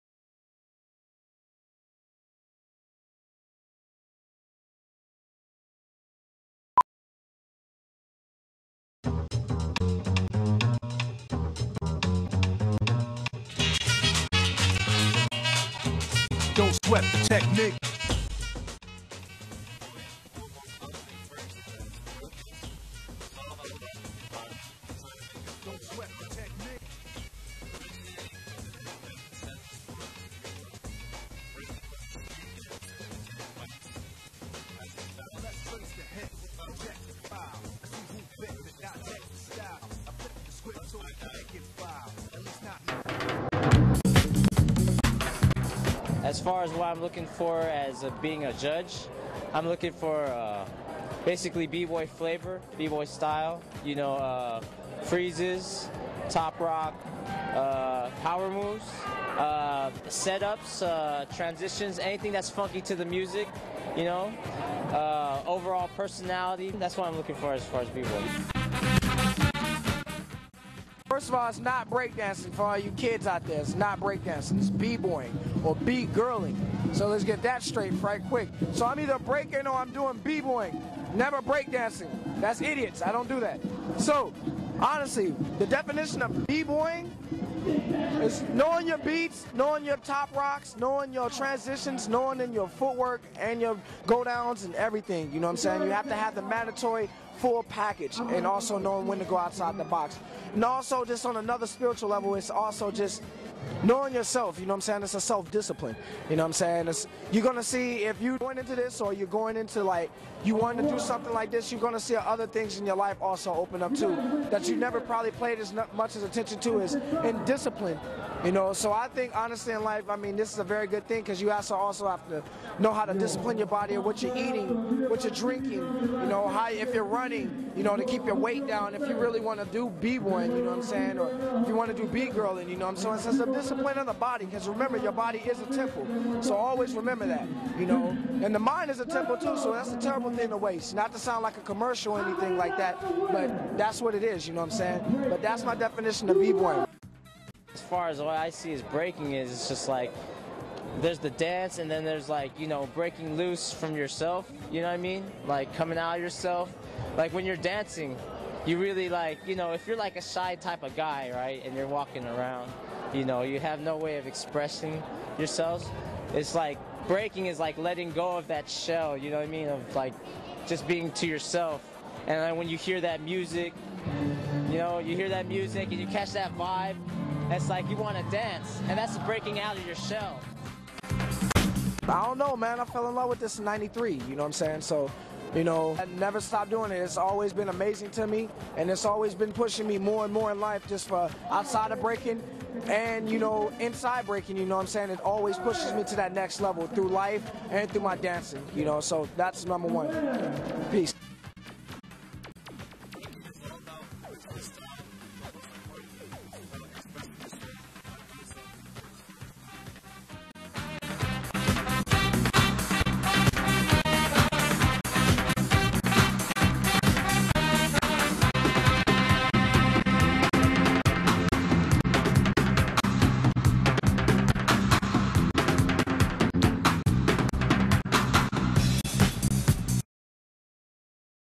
Breakdancing?
Really short high school documentary I made on bboyin a couple years ago at Riordan's Breakdown event
Yes, bad voice-over in final cut pro...:( you can still make do with machine's and rob nasty's interviews thats where most of the content is anyways